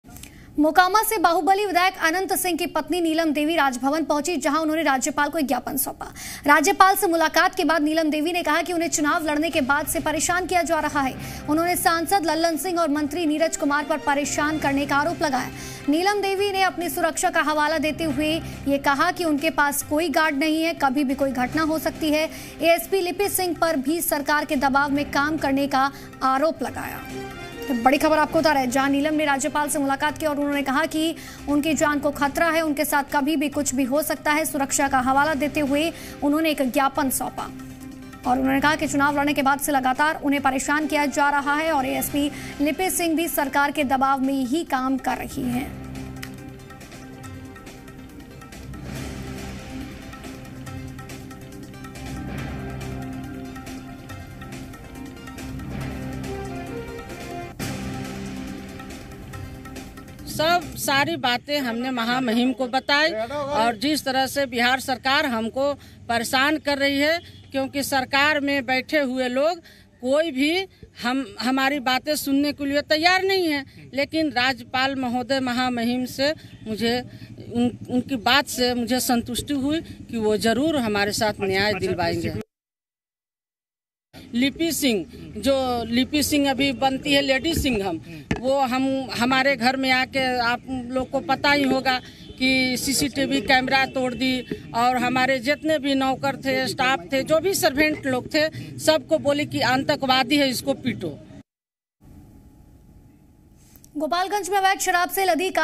पटना- राज्यपाल से मिली अनंत सिंह की पत्नी नीलम देवी.
राज्यपाल से मिली अनंत सिंह की पत्नी नीलम देवी, चुनाव लड़ने के बाद से हमें परेशान किया जा रहा- नीलम देवी.